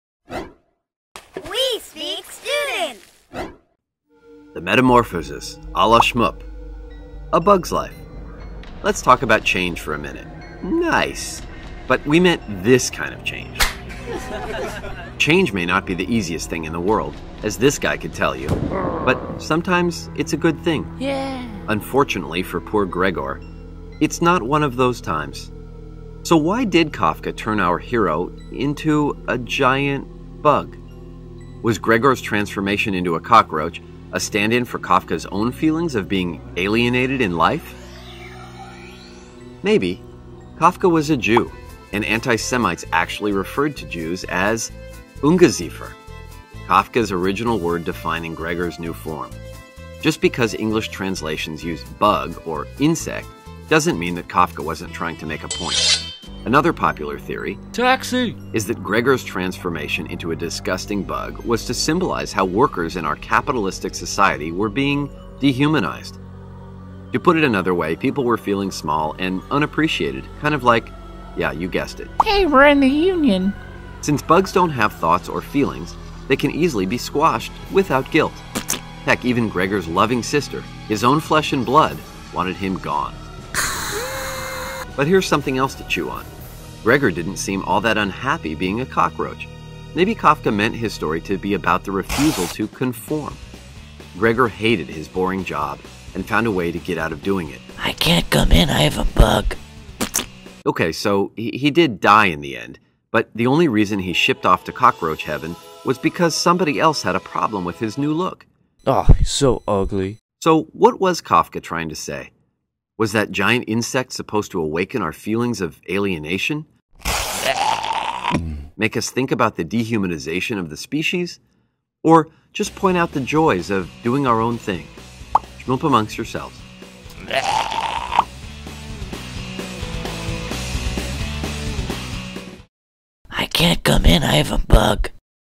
The Metamorphosis by Shmoop
We've had bad dreams before but waking up as a giant cockroach? That's a new one. That's what happens in Kafka's the Metamorphosis. Oh well, maybe it wouldn't be so bad. They have been around for a million years -- they must know something we don't.

Come check out shmoop guide for Metamorphosis.